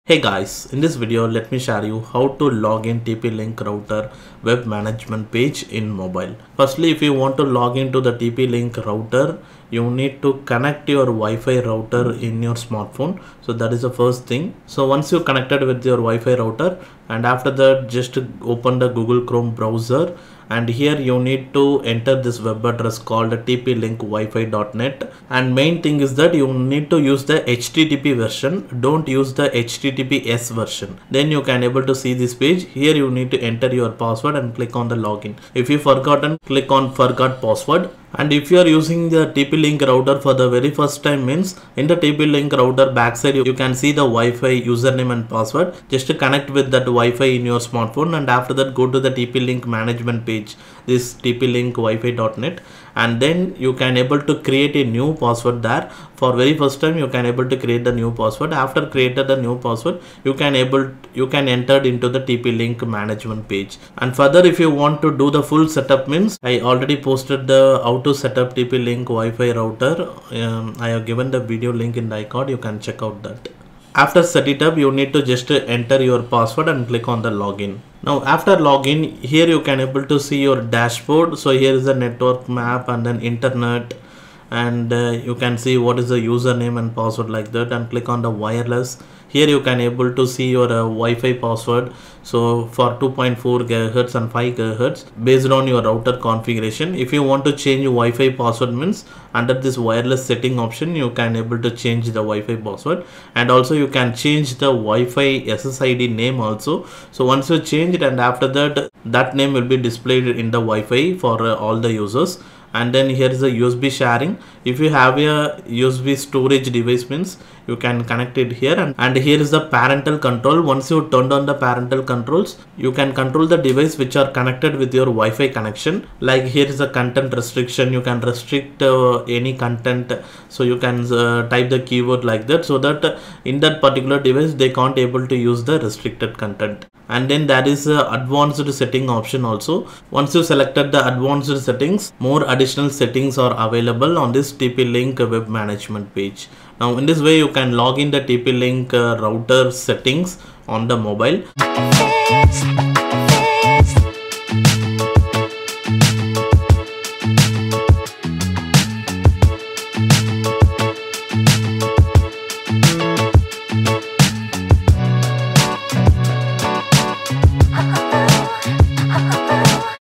How to login tp link router in mobile - TP Link Web Management Page
Let me share the tutorial on how loging tp link router in mobile, once logged in tp link management page you can change wifi name, password and other advanced settings of tp link router.

Best TWS Earbuds | Rs. 2000

Best Tws Earbuds | Rs. 1500

Best TWS Earbuds | Rs. 5000